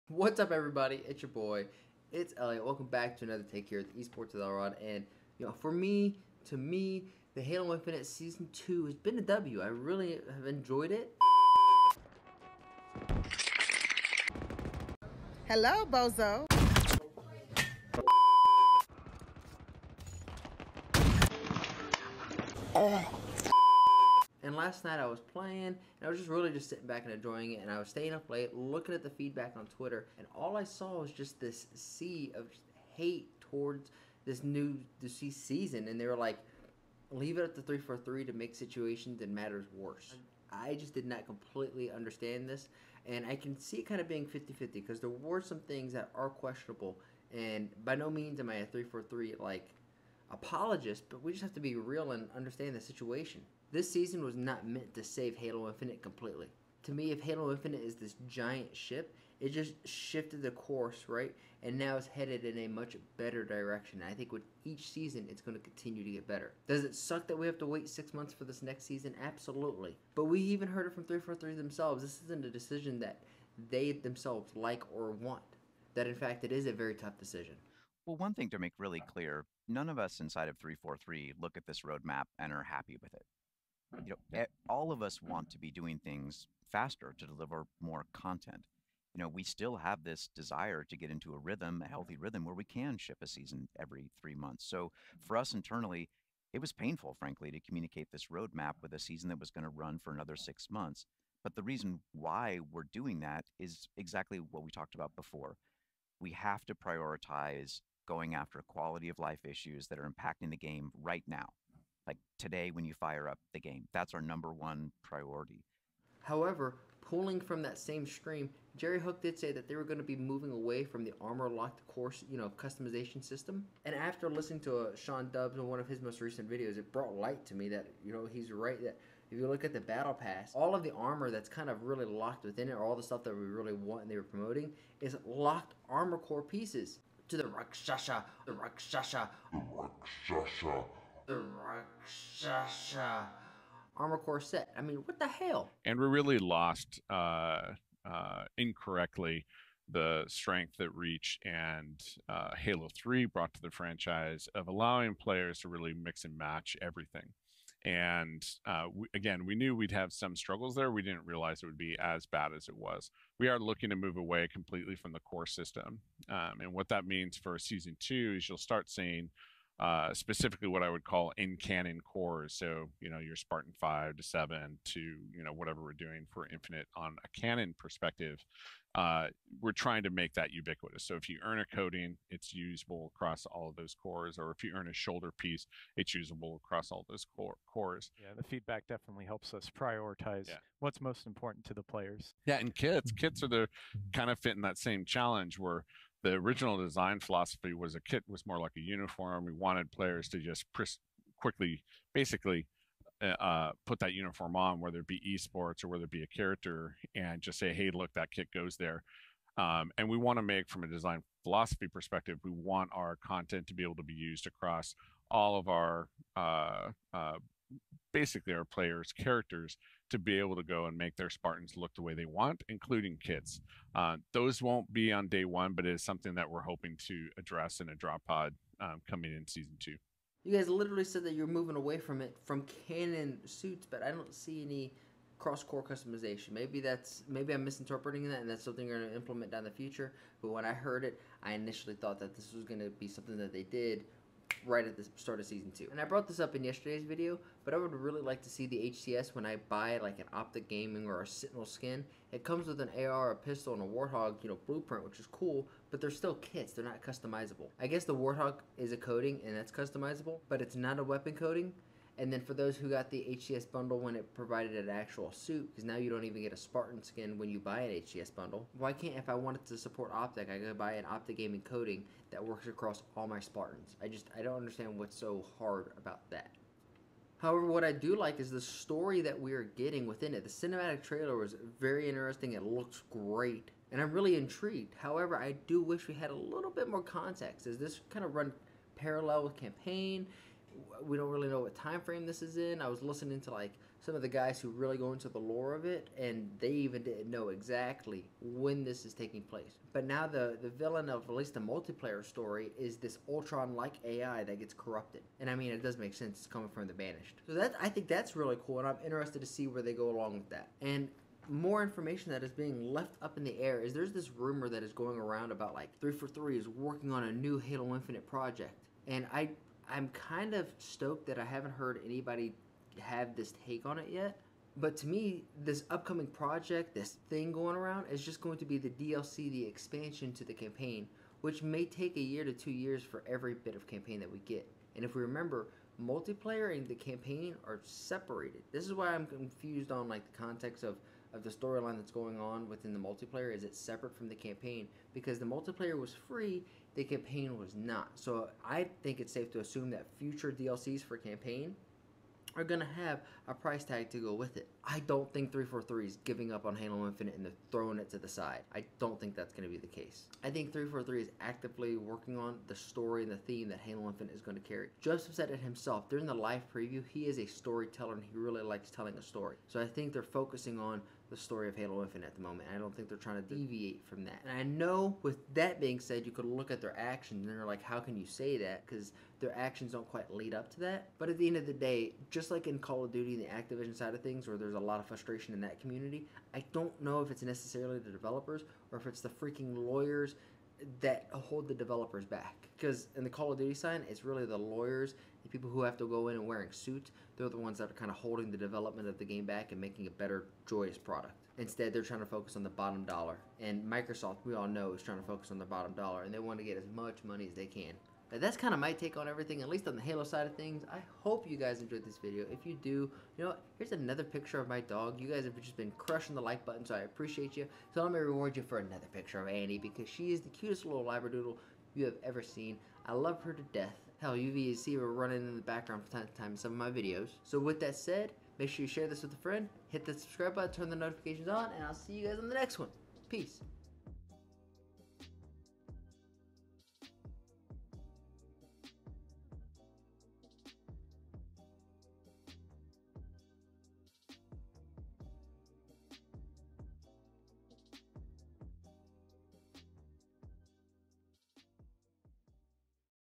Halo Infinite & Call of Duty SHARE THIS PROBLEM!
Halo & Call of Duty Share this problem. They both are held back by the Lawyers & “Suits” that dictate a lot of what the developers can do. I TRULY BELIEVE THE INCOMPETENCE COMES FROM THEM NOT THE DEVELOPERS. The ones who know how to pair the “1”s and “0”s to get a grenade to bounce off the ground in a certain direction have the capacity to build a wonderful game!

For me, Halo Infinite Season 2 has been great. I love the new modes, I love the new maps & cosmetics. I think if Halo is a ship, this shifted the course and we are headed in a better direction. With each season, working our way to become the #1 FPS game in the Industry. I believe Halo has the potential for that.

However, as a community we NEED TO CHILL ON THE DEVELOPERS - Just a little bit - but that doesn’t mean we can’t give any feedback or critics. During the most recent live stream we were told that in season 2 we would be getting the ability to use a coating across all cores, weapons & vehicles yet when we look at the battle pass. That doesn’t actually seem to be the case! Holding them to their word is something we should be doing.

What critics, complaints do you have about season 2? I know I am opening a can of worms by asking this but I am curious to see ya’lls feedback!

*ALSO, sorry for the late update!*